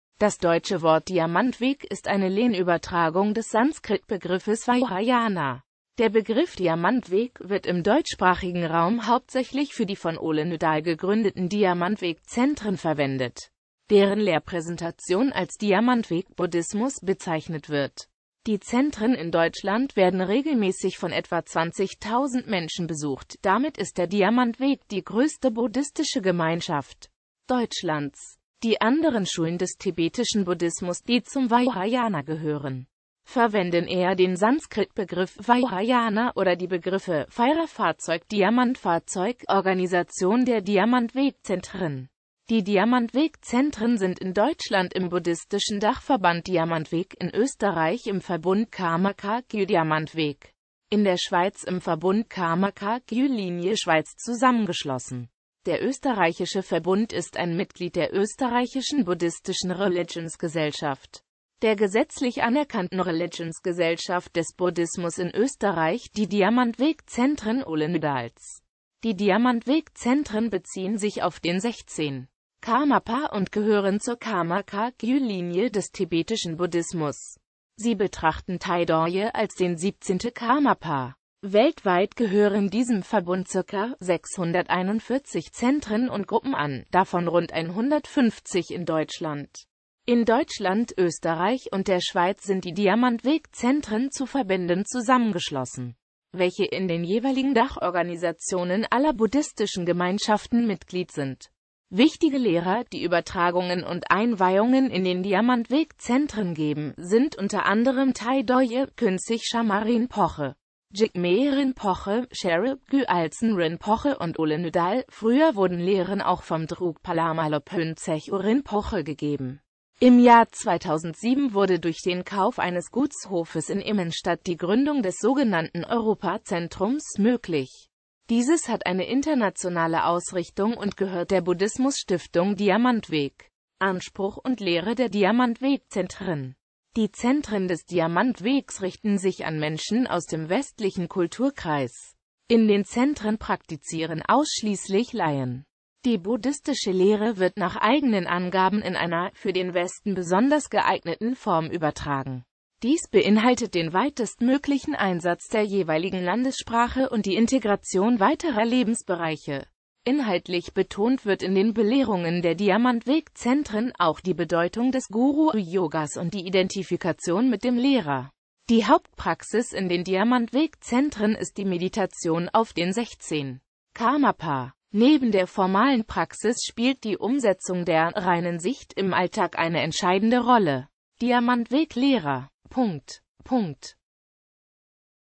Diamantweg
Vielen Dank für Ihre Unterstützung:

Das deutsche Wort Diamantweg ist eine Lehnübertragung des Sanskritbegriffes vajrayāna .Der Begriff „Diamantweg“ wird im deutschsprachigen Raum hauptsächlich für die von Ole Nydahl gegründeten Diamantweg-Zentren verwendet, deren Lehrpräsentation als „Diamantweg-Buddhismus“ bezeichnet wird.Die Zentren in Deutschland werden regelmäßig von etwa 20.000 Menschen besucht – damit ist der Diamantweg die größte buddhistische Gemeinschaft Deutschlands.Die anderen Schulen des tibetischen Buddhismus, die zum Vajrayana gehören, verwenden eher den Sanskritbegriff „vajrayāna“ oder die Begriffe „Vajra-Fahrzeug“, „Diamantfahrzeug“.

Bild-Copyright-Informationen
Urheber Info: Lisandrom